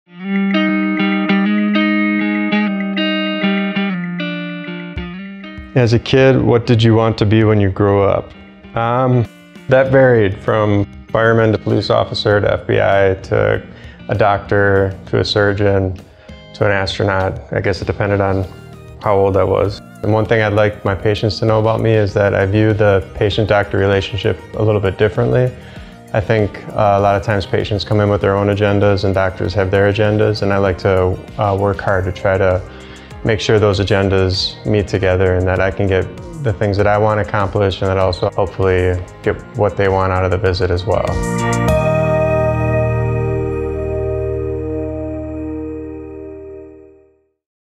Meet Our Expert - Christopher Mueller, MD
Christopher Mueller, MD, is a cardiologist at Northern Arizona Healthcare’s Cardiovascular Institute in Flagstaff, Arizona.